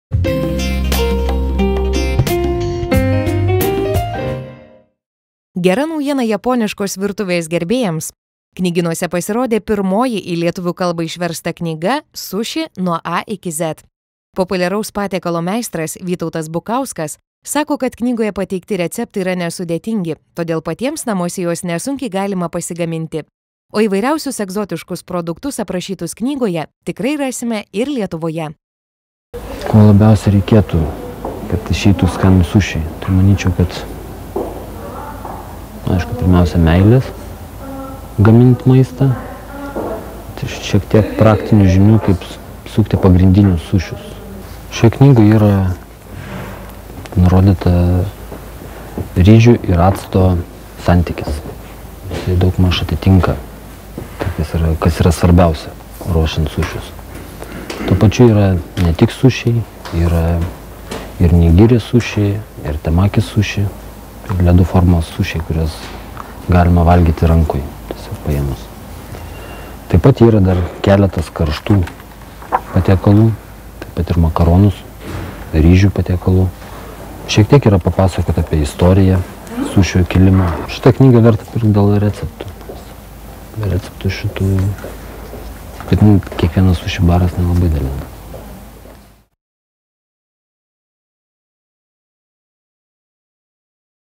„Sushi. Nuo A iki Z" (Sud. Raminta Nagelytė, „Obuolys", 2011 m.)
Gera naujiena japoniškos virtuvės gerbėjams. Knygynuose pasirodė pirmoji į lietuvių kalbą išversta knyga „Sushi. Nuo A iki Z".  Populiaraus patiekalo meistras, Vytautas Bukauskas sako, kad knygoje pateikti receptai yra nesudėtingi, todėl patiems namuose juos nesunkiai galima pasigaminti. O įvairiausius egzotiškus produktus, aprašytus knygoje, tikrai rasime ir Lietuvoje.